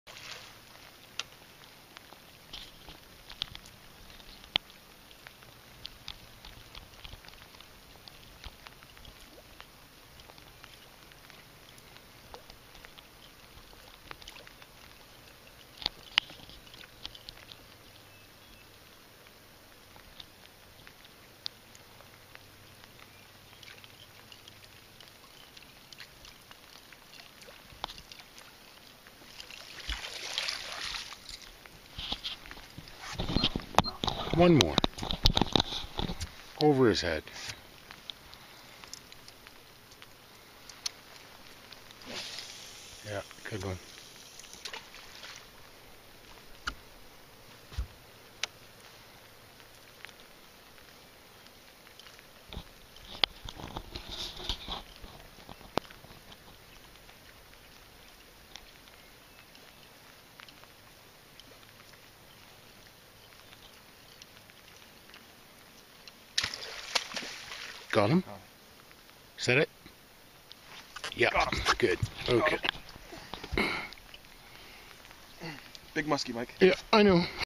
French River Topwater SKI 2014
Great video from last year at the French River. My brother in Laws Chris & Mike have turned into top shelf Musky Hunters.

Wait for it... Wait for it.